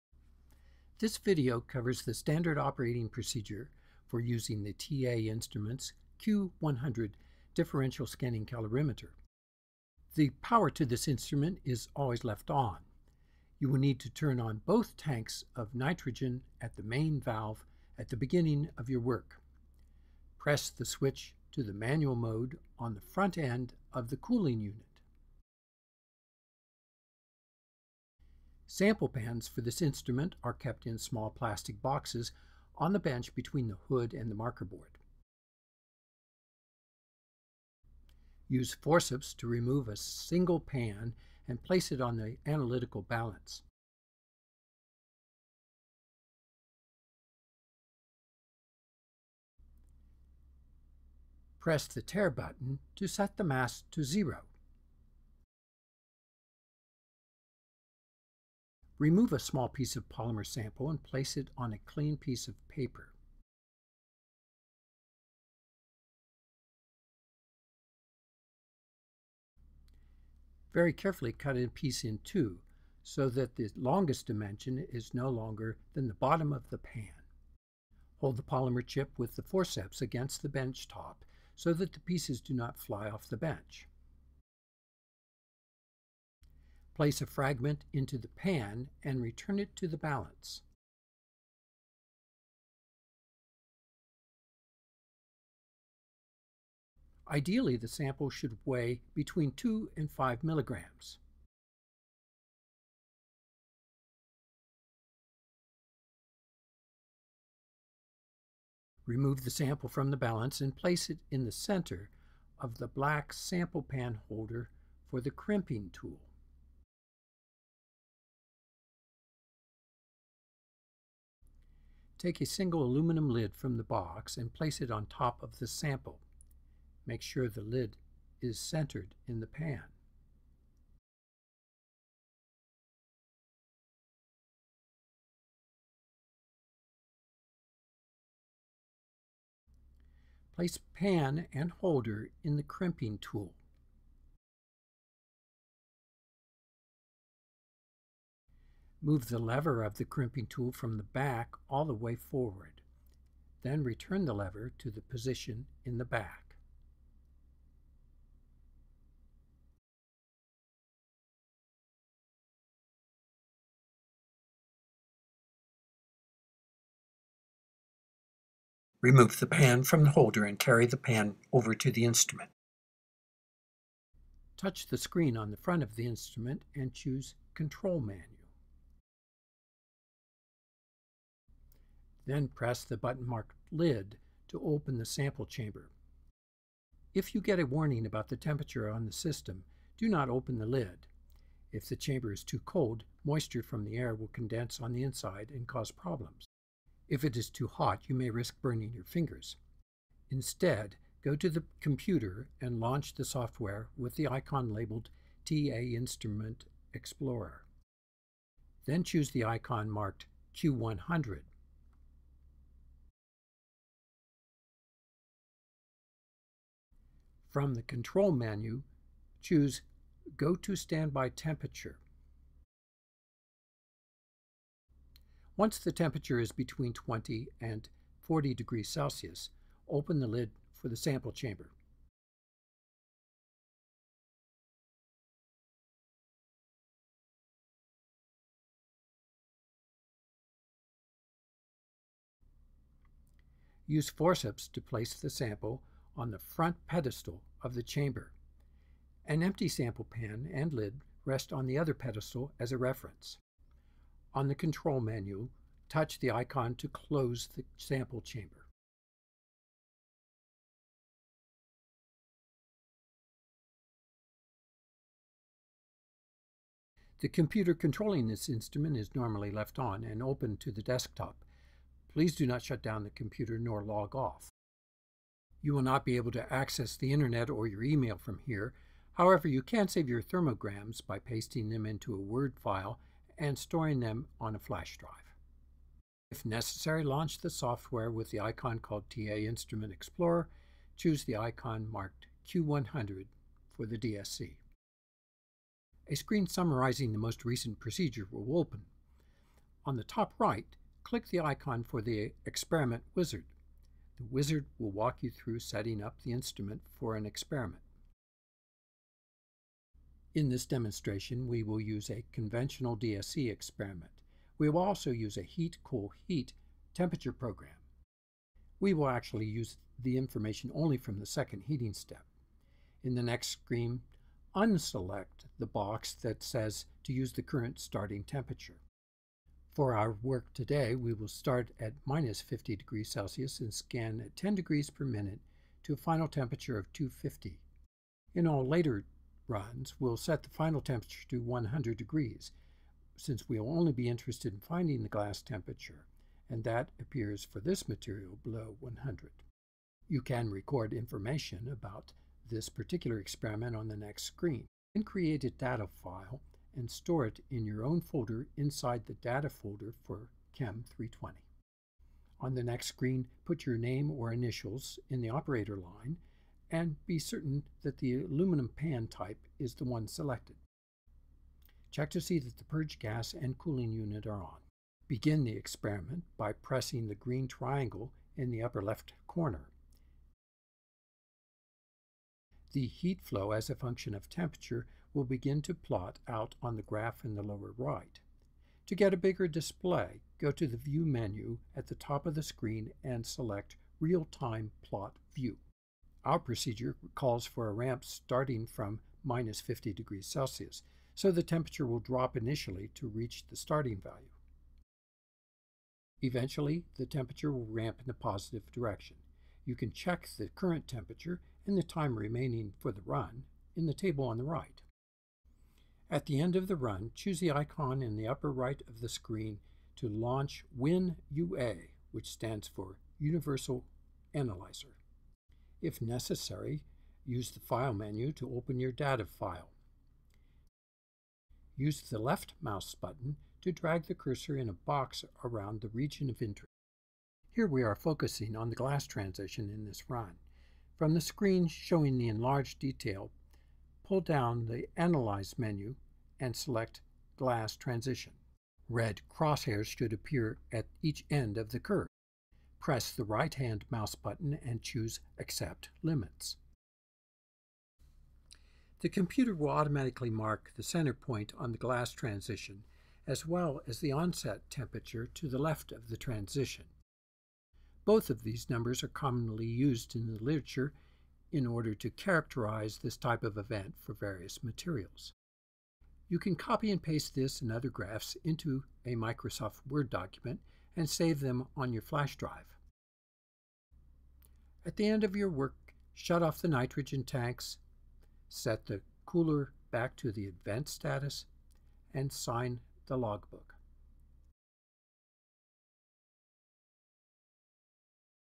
Q100 DSC standard operating procedure
This lesson covers the standard operating procedure for TA Instrument’s Q100 Differential Scanning Calorimeter (DSC).